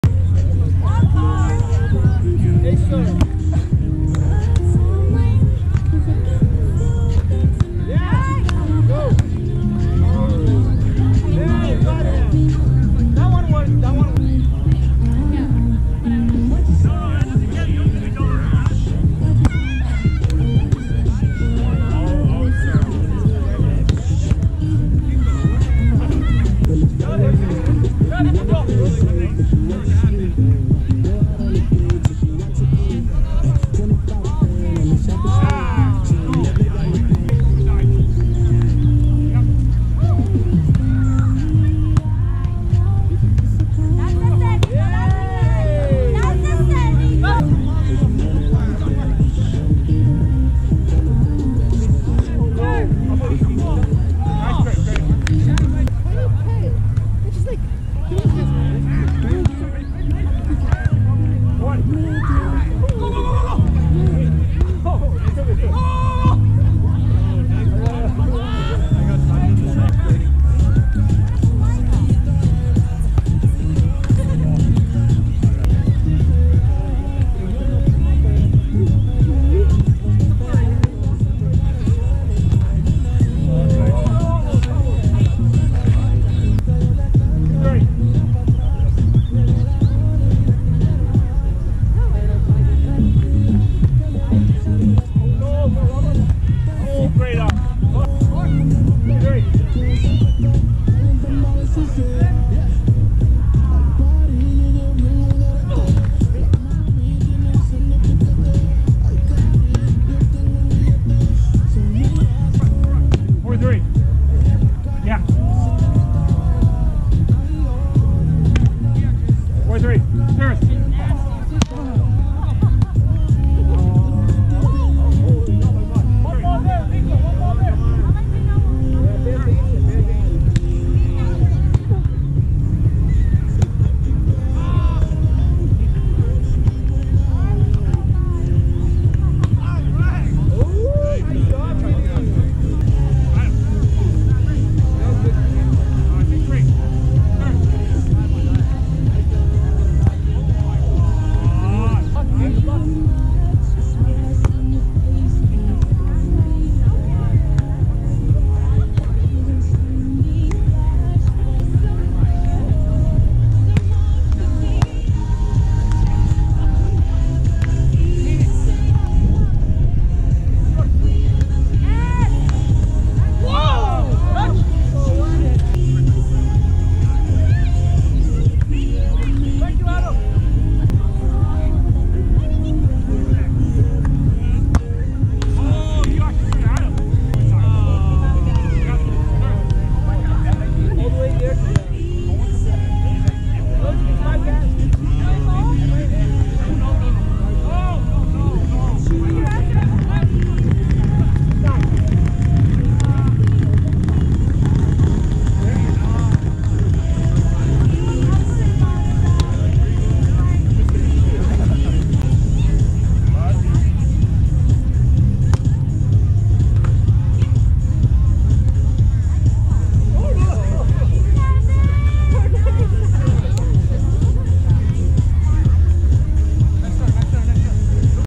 WBV Kev/Nico vs Adam/Vinny 6/6/23 Game 1
Kev/Nico vs Adam/Vinny
Partial Game (only caught part of game because of taping of mavericks game previously)